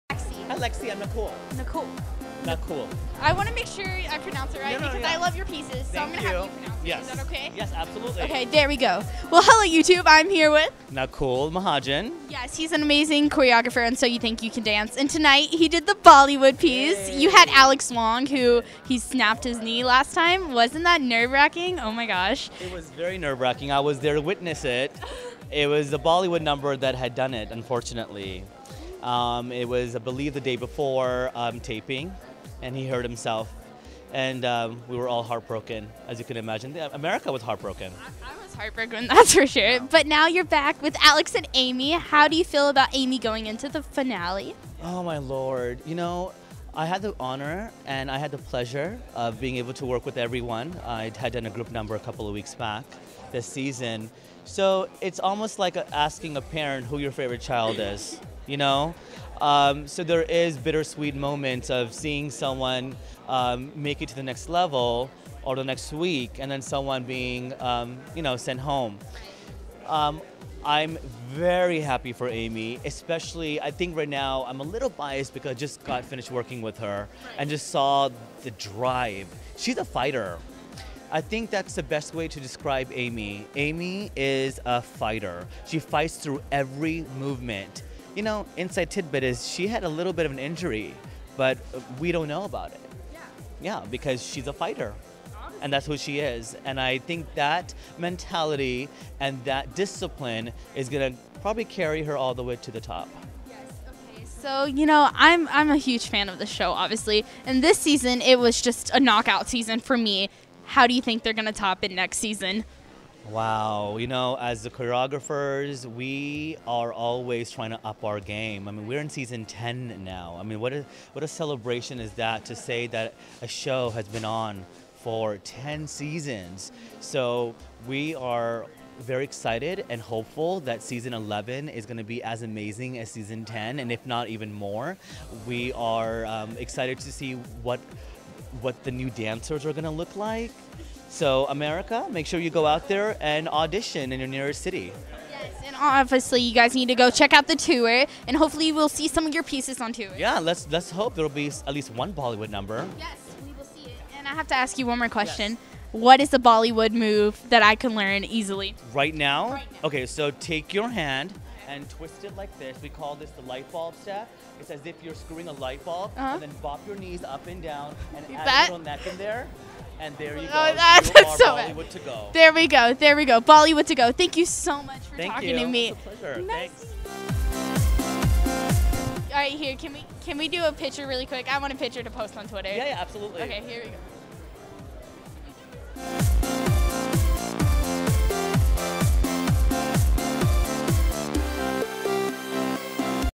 NAKUL MAHAJAN SO YOU THINK YOU CAN DANCE INTERVIEW AUG 27! SYTYCD NAKUL MAHAJAN
Teen Reporter- Lexi DiStefano
Editor - T. Patrick Johnson